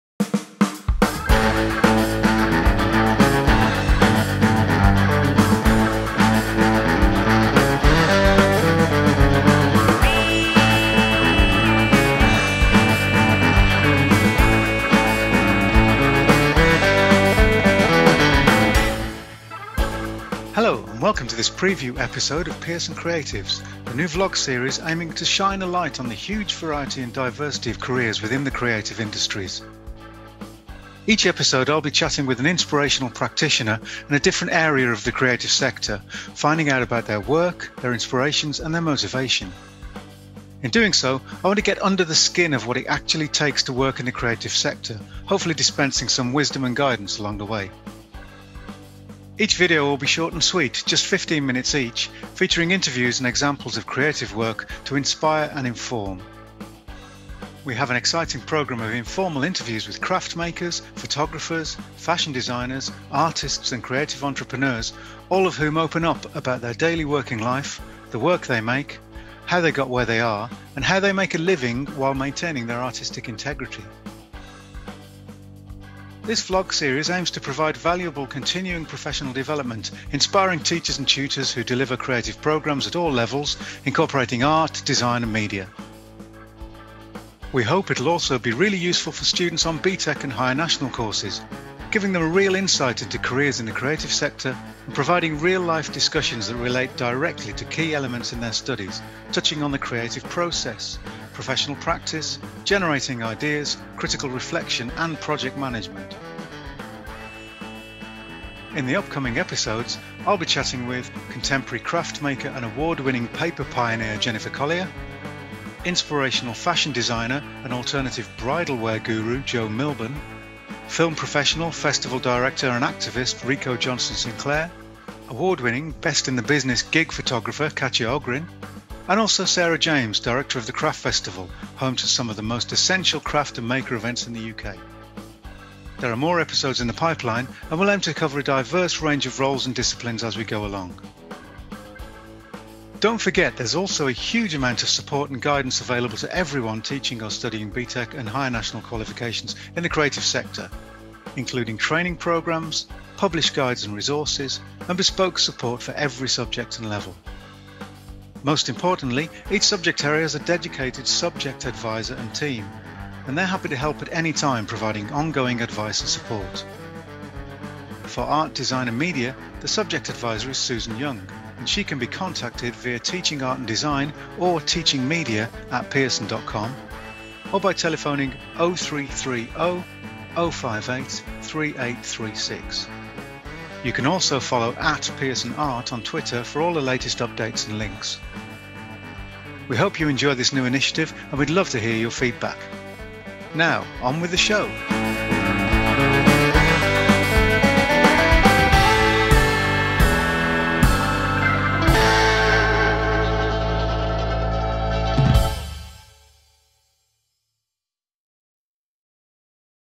Pearson Creatives Art, Design and Media Vlog Series
Join Senior External Examiner and BTEC Trainer Simon Fox in his brand new vlog series, aiming to shine a light on the huge variety and diverse range of careers within the creative industries. Each episode Simon will be chatting with an inspirational practitioner in a different area of the creative sector to find out about their work, inspirations and their motivation.